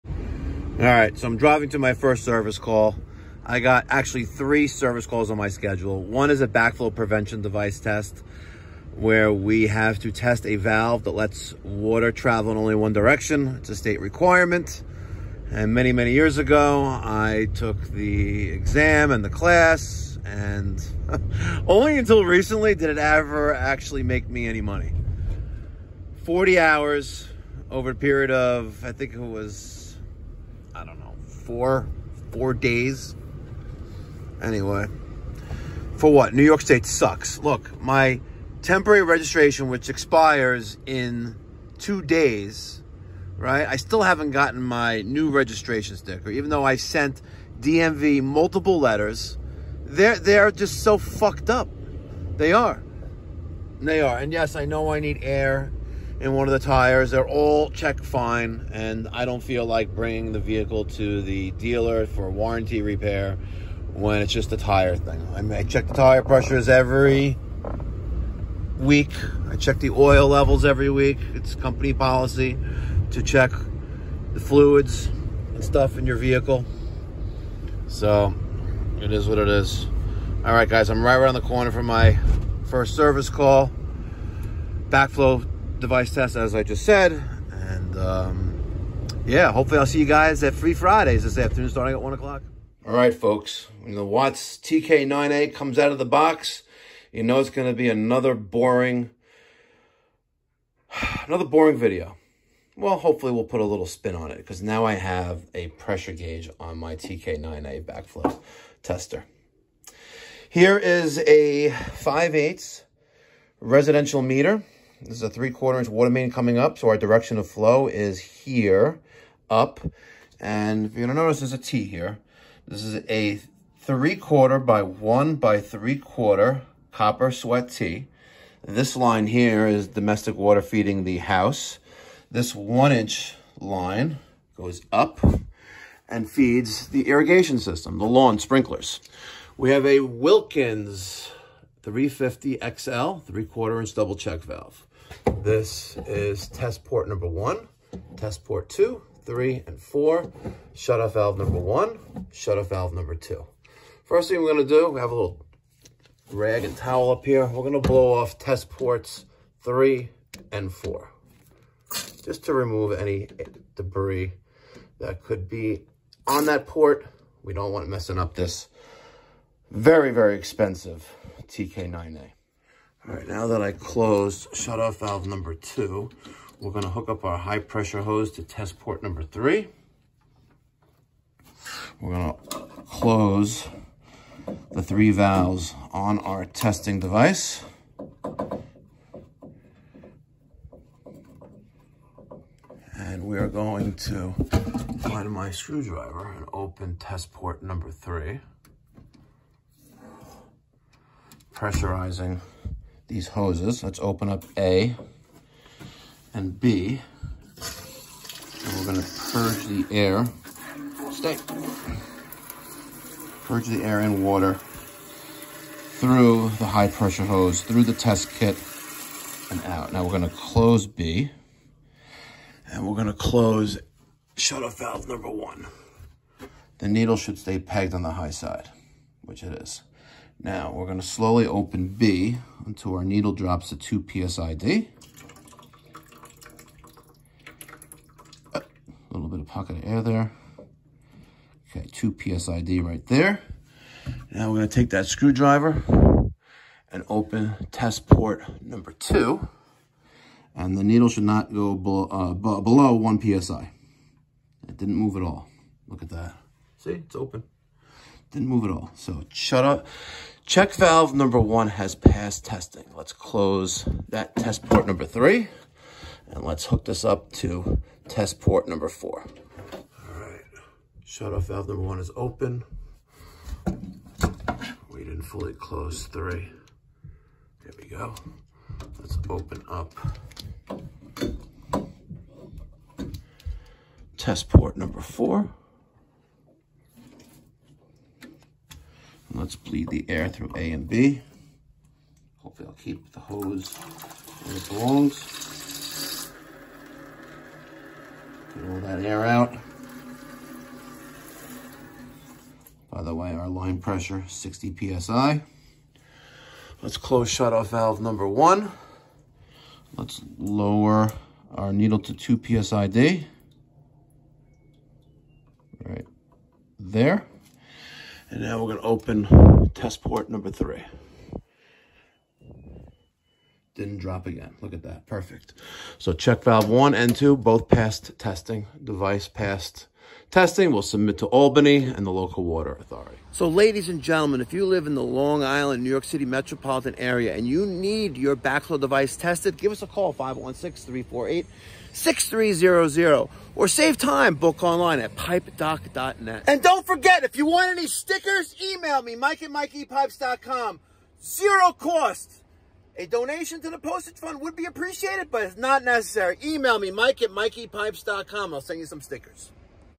Residential Backflow Test – Step-by-Step with Wilkins 350XL & Watts TK-9A
💦 Residential Backflow Prevention Device Tested in Real Time!

Join Mikey Pipes on a real-world service call as we perform a required New York State backflow test on a residential irrigation system using a Watts TK-9A testing device and a Wilkins 350XL double check valve.

Learn the step-by-step procedure for testing both check valves, checking PSID, bleeding air, and confirming proper operation — all with that Mikey Pipes edge 🔧💪

🚨 Did you know? Backflow testing is mandatory in many municipalities to keep your drinking water safe from contamination!

👉 Serving Long Island & NYC Metro? Need a backflow test?

⏱️ Video Chapters
00:00 - Driving to First Service Call
00:48 - Intro to Backflow Testing & TK-9A Tester
01:40 - System Layout: Water Main, Meter, and Wilkins 350XL
02:45 - Purging Air from Ports 3 & 4
03:25 - High Side Hose Setup and Pressurization
04:10 - Testing Check Valve #1
05:05 - Testing Check Valve #2
06:00 - Line Pressure Reading & Final Results
06:35 - Call to Action: Book Your Backflow Test